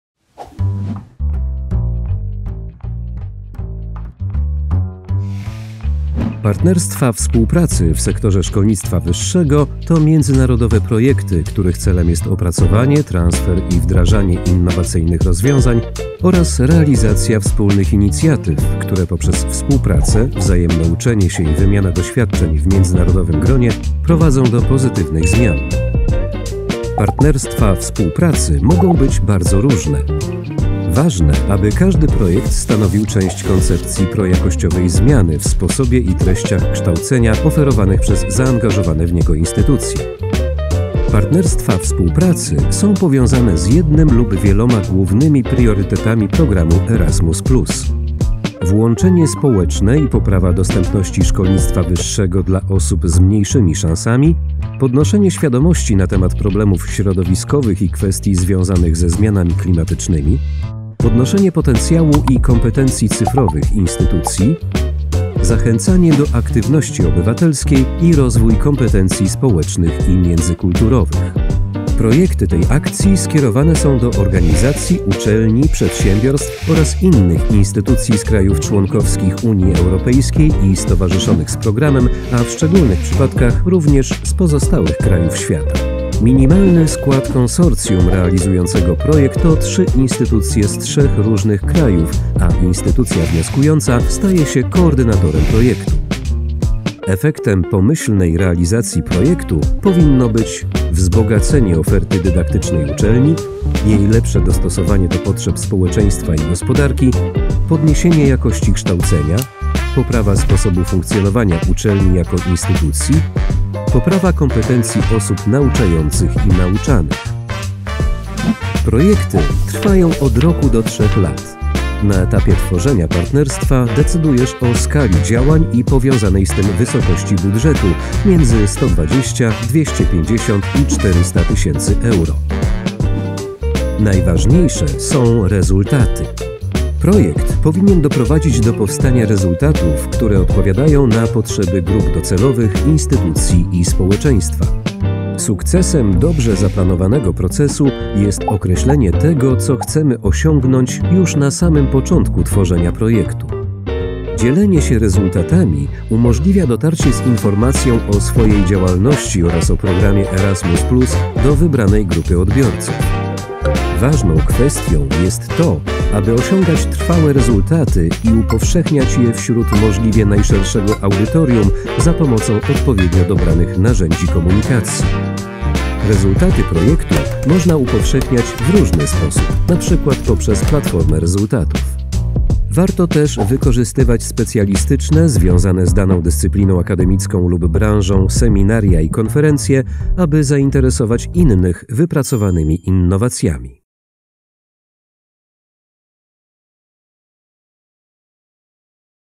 Partnerstwa współpracy Erasmus+ dla szkolnictwa wyższego
Partnerstwa współpracy” w sektorze szkolnictwa wyższego to międzynarodowe projekty, których celem jest opracowanie, transfer i wdrażanie innowacyjnych rozwiązań oraz realizacja wspólnych inicjatyw promujących współpracę, uczenie się i wymianę doświadczeń na poziomie europejskim.